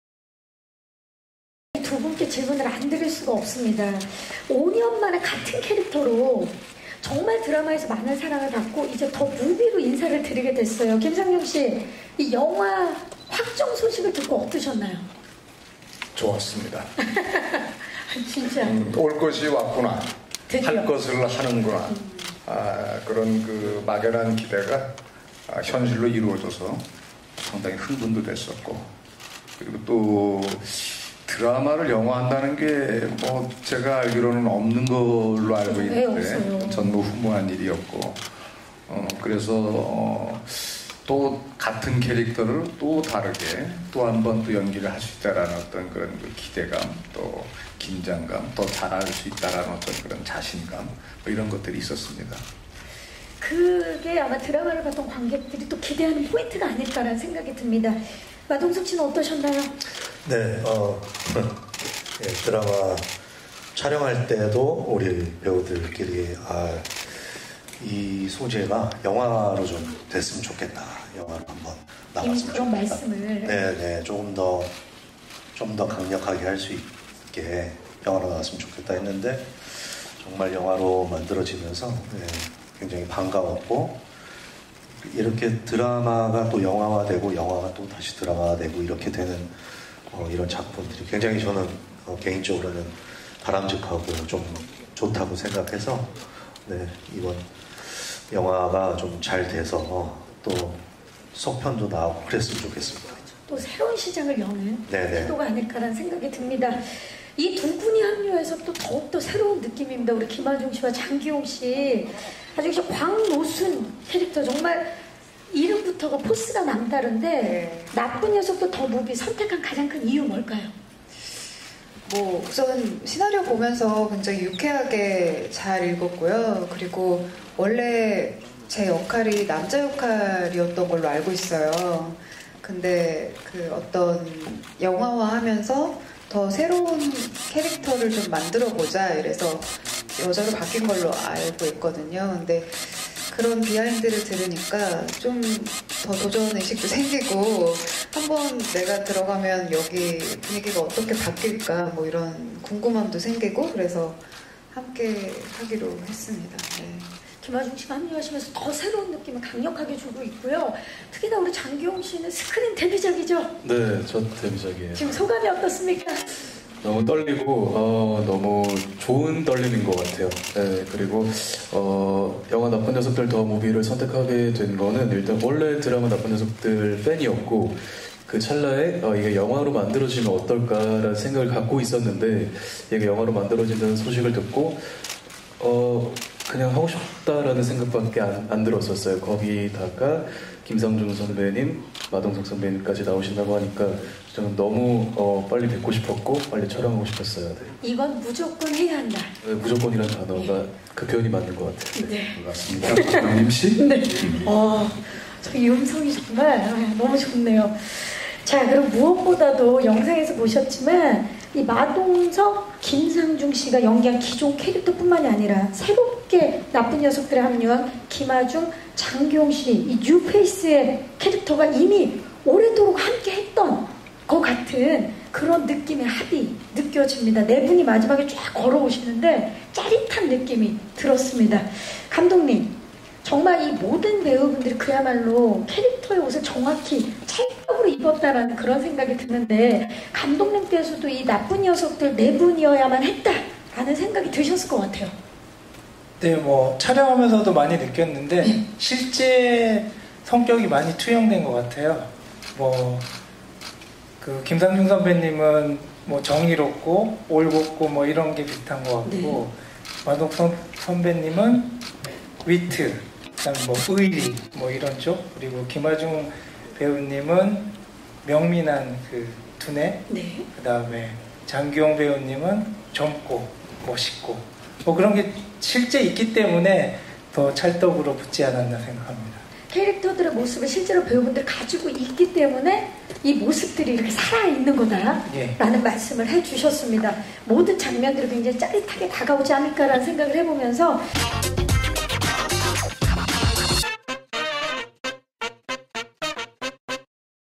190729 배우들의 캐스팅 소감 & 감독의 캐스팅 이유 ('나쁜녀석들 더무비' 제작보고회)
29일 오전 서울시 강남구 신사동 압구정CGV에서 영화 '나쁜녀석들:더무비'의 제작보고회가 열렸다. 현장에는 배우 마동석, 김상중, 김아중, 장기용, 손용호 감독이 참석해 머그샷 포토타임을 갖고 있다.

영화 '나쁜녀석들:더무비'는 사상 초유의 호송 차량 탈주 사건 발생 후, 사라진 최악의 범죄자들을 잡기 위해 다시 한번 뭉친 나쁜 녀석들의 거침없는 활약을 그린 범죄 오락 액션으로 9월 개봉한다.